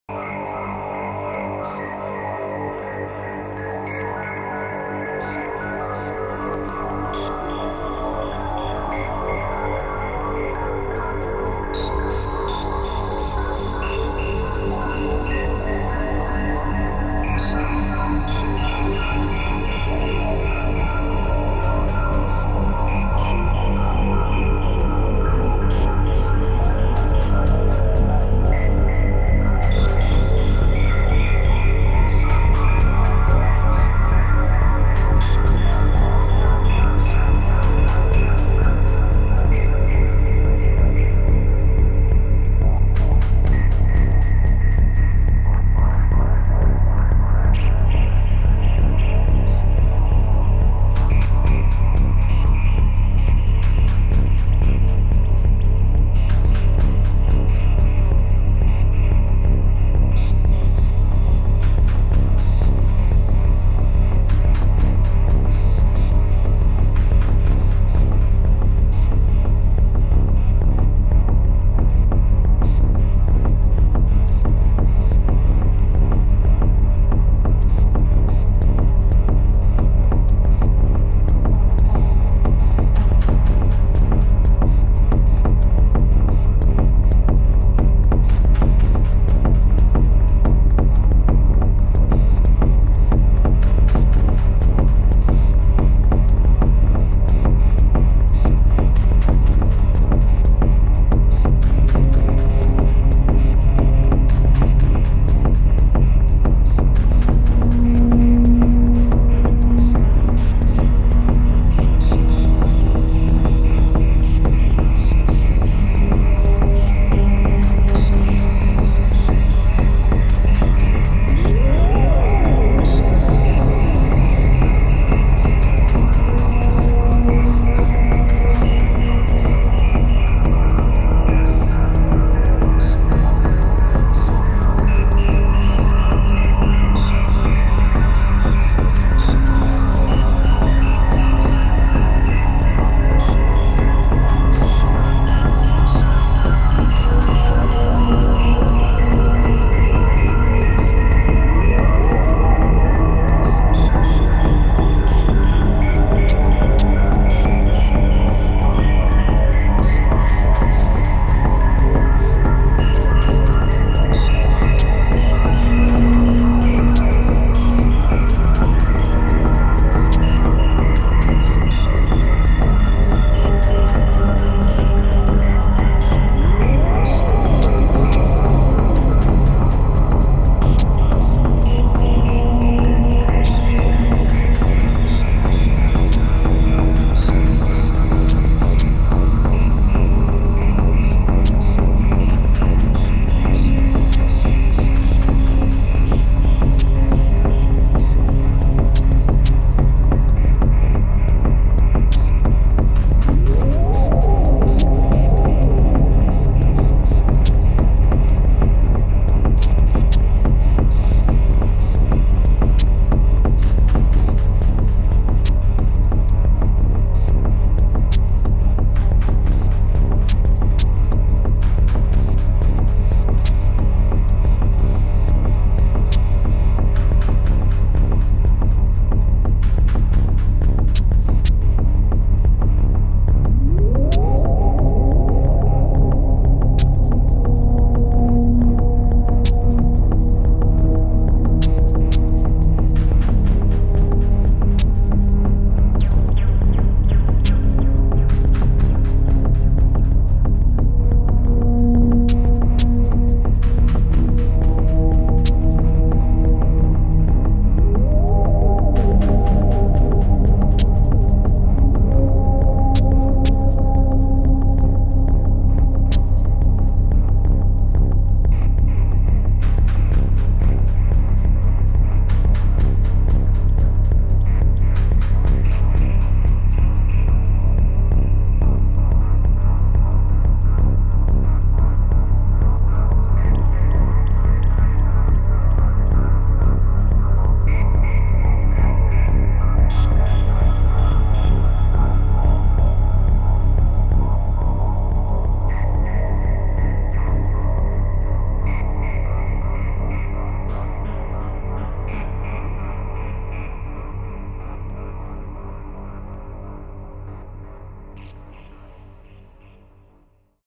46 - New summer Moog video
The powerful Moog Mother 32 for impro melody, the evocative pads of Piston Honda, and the hypnotic rhythms of the Elektron Analog Rytm, immerse yourself in an epic journey of experimental techno. The art of modular improvisation comes to life, crafting unprecedented soundscapes.